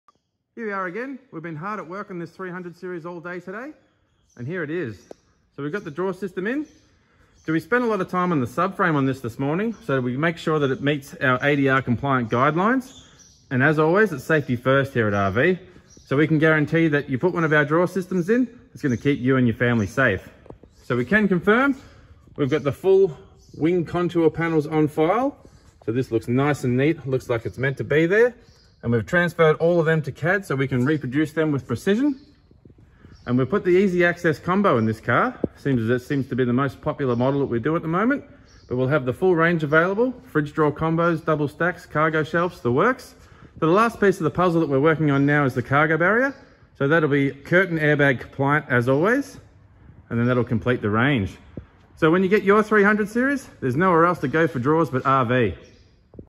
300 Series
Once again our extremely talented engineer, Hayden Oliver, has finalised the "FIRST EVER 300 SERIES 4WD DRAWERS & ACCESSORIES."
This 300 series is fitted with a Premium Alloy Easy Access Combo-1 1000L x 1020W x 275H and Fridge Slide-1(RV-FS-1).
The Drawers and Accessories are as always available in Premium Steel, Premium Alloy and EcoLite options and compatible with all RV Accessories. The RV Storage Solutions products are built by Australians in Australia.  Check out the video and get in touch with us for more information on how to deck out your 300 Series. Taking orders NOW!